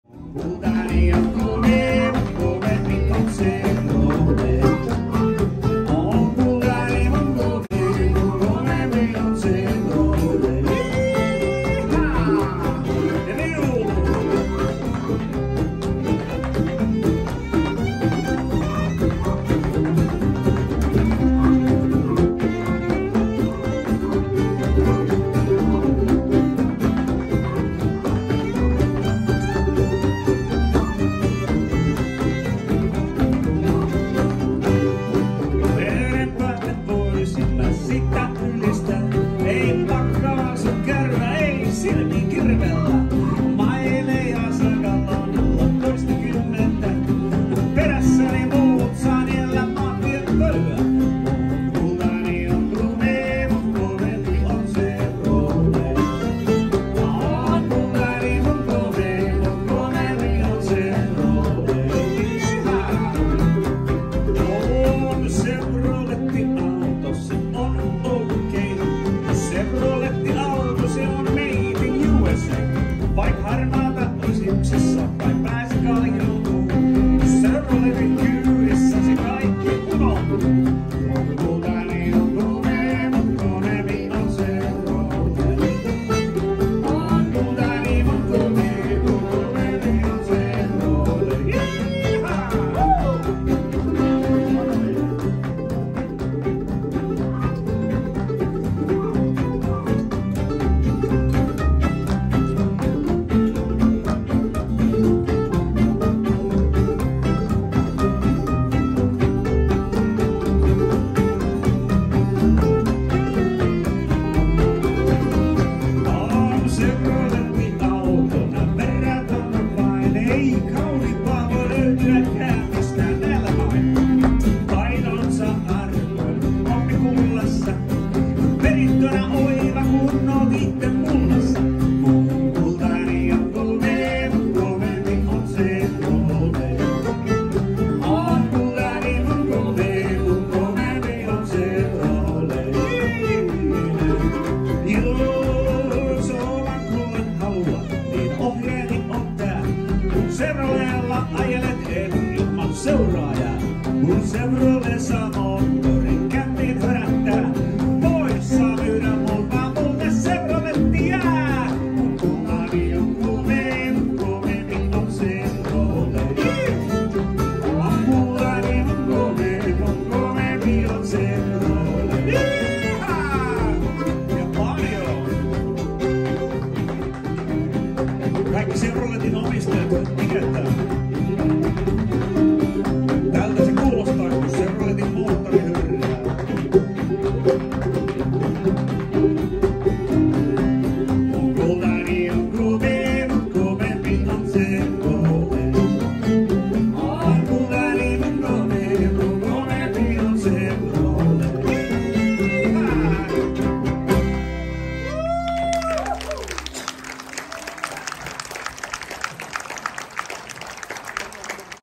Buckdancing Vlog - Bluegrass Sauna Baptism - Miss Moonshine w Wasel & Weasels in Finland
when bluegrass and sauna come together